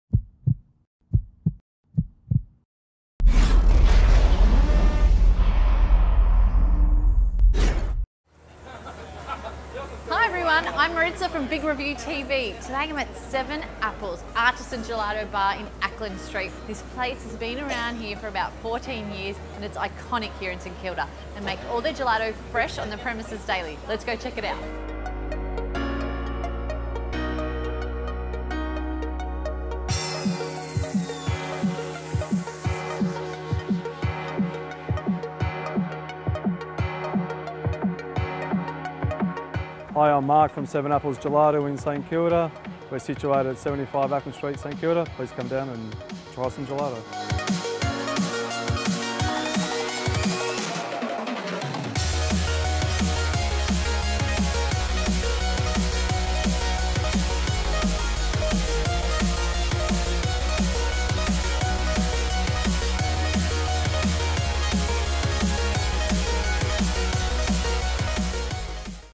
7 Apples Gelato, an Ice Cream Shop in Melbourne serving Italian Ice Cream and Sorbet
7 Apples Gelato is located Melbourne VIC. Gelato and vegan sorbets made fresh daily. 7 Apples Gelato sources only the finest and freshest ingredients. 26 flavours available at any one time with about 100 flavours in all.

7 Apples Gelato
75 Acland St, St Kilda VIC Australia 3182